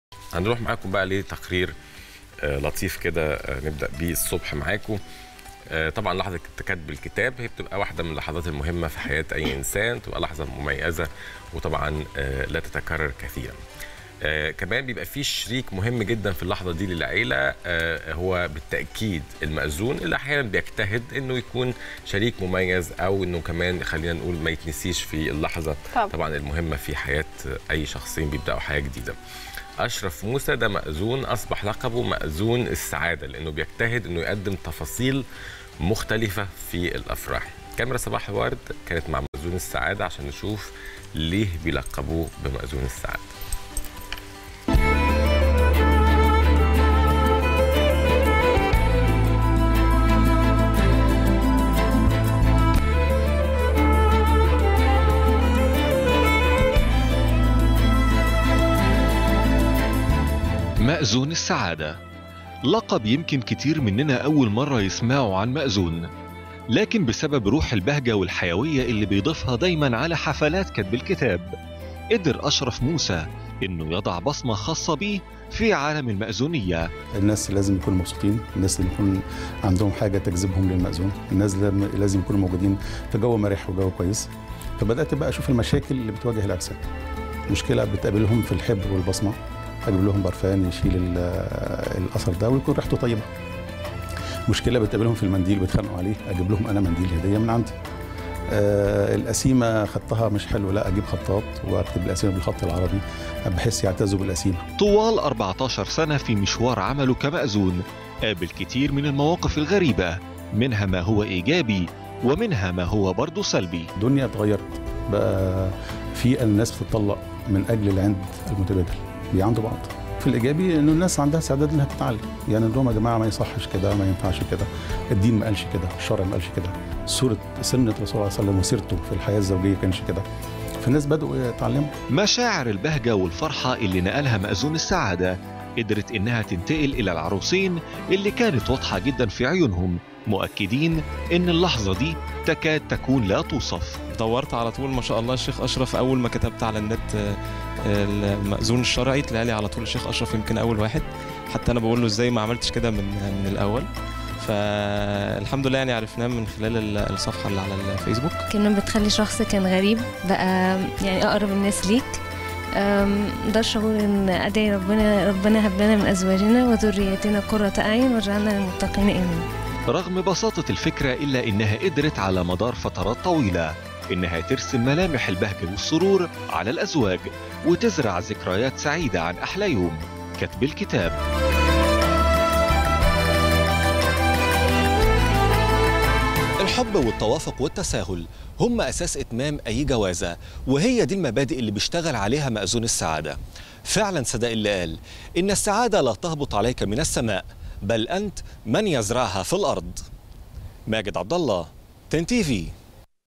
مأذون السعادة.. يصنع لحظة مميزة للعروسين بأسلوبه الخاص
يمكننا أن نصف عمل "أشرف موسى" المأذون الشرعى الذى يلقب بـ"مأذون السعادة" بسبب روح البهجة والحيوية التى يضفيها على حفلات كتب الكتاب، وأيضًا بسبب تلك النقلة فى حياته من عالم الفندقة إلى المأذونية ليضع بصمة مختلفة على مهنته.